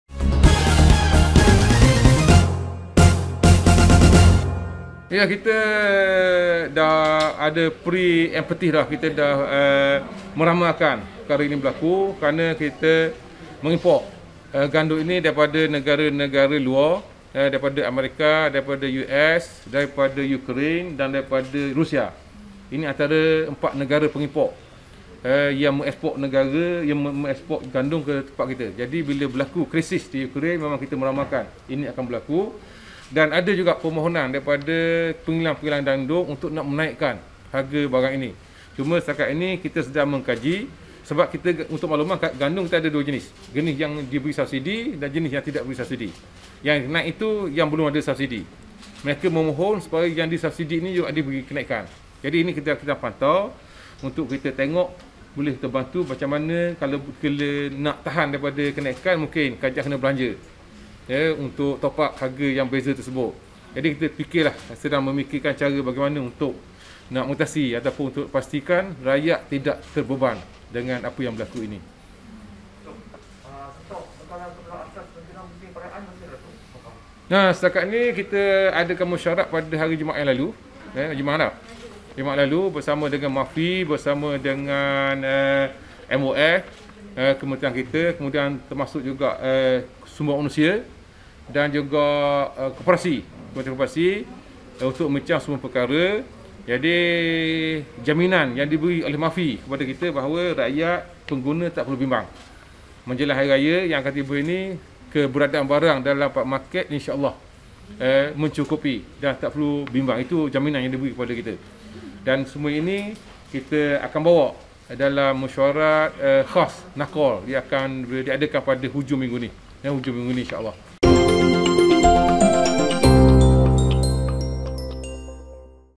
MGNews : Kerajaan Sudah Ramal Kenaikan Harga Gandum - Rosol Wahid
Kementerian Perdagangan Dalam Negeri dan Hal Ehwal Pengguna (KPDNHEP) sudah membuat ramalan akan adanya kenaikan harga komoditi barangan seperti gandum sejak berlakunya krisis besar antara Ukraine dan Rusia ketika ini.